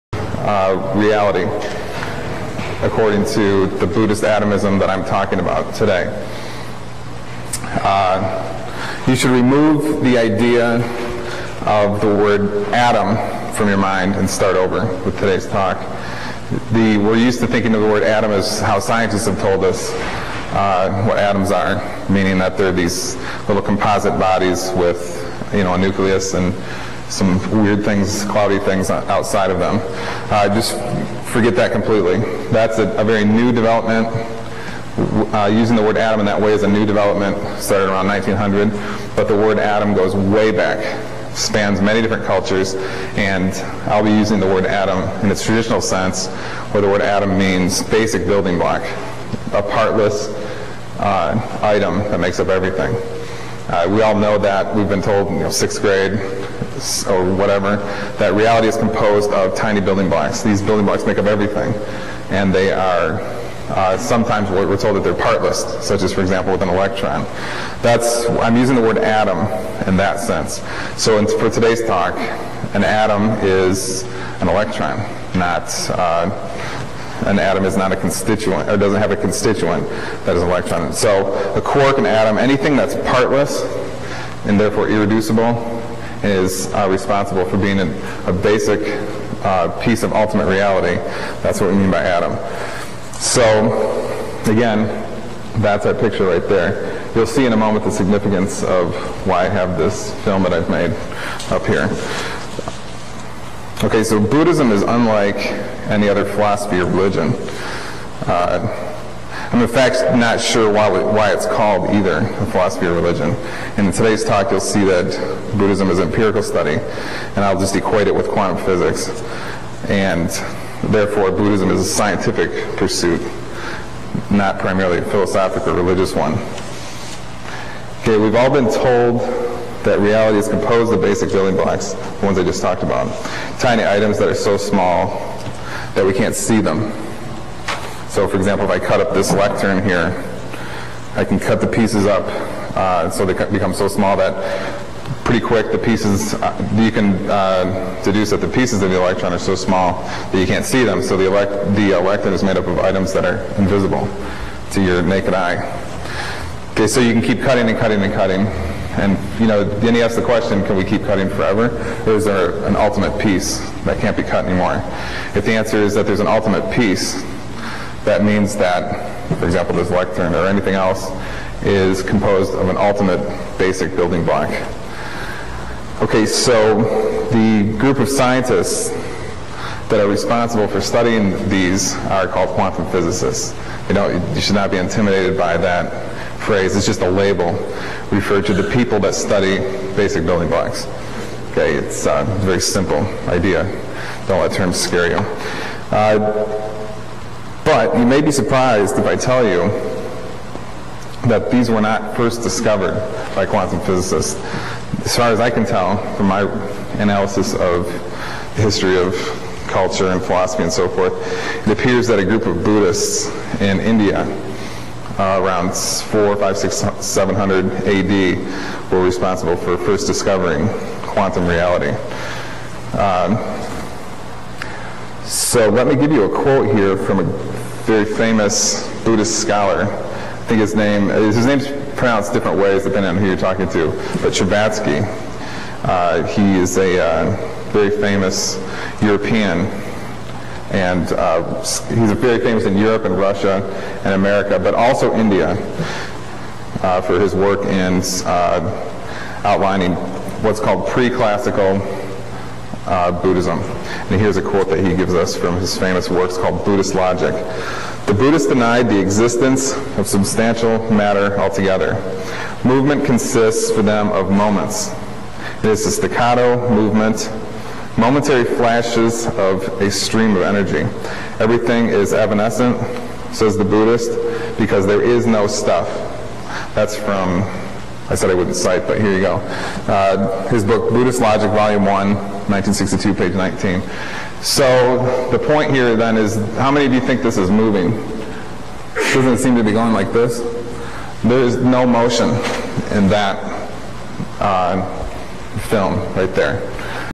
Buddhism and Quantum Physics -1
Jeffrey Grupp is a Buddhist philosopher, a political philosopher, and a lecturer at Purdue University Calumet and Purdue University Northcentral, both of which are in Northwest Indiana. He is the author of Corporatism (published by Progressive press, which is also Webster Tarpley's publisher) and The Telescreen {not yet published). He has published twelve articles in academic philosophy journals (Metaphysica, Indian International Journal of Buddhist Studies, Disputatio, among others), and he is a regular guest on the Nutrimedical Report, which is Dr. Bill Deagle's radio show. Grupp is the former associate editor of the prestigious academic philosophy journal, PHILO (editor: Quentin Smith). Grupp is the originator of the mereological nihilist interpretation of quantum mechanics (published in Axiomathes in 2006), and he has written about issues to do with domination by global corporations around the world. He has two web sites

Grupp has said on national radio that his political views are entirely absent from his university teaching and his political philosophy and academic teaching do not ever mix because universities would not consider his political positions real or credible information. Grupp's academic work in mainstream philosophy (specifically metaphysics, Buddhism, and quantum mechanics) has been claimed by Quentin Smith to be original and rigorous.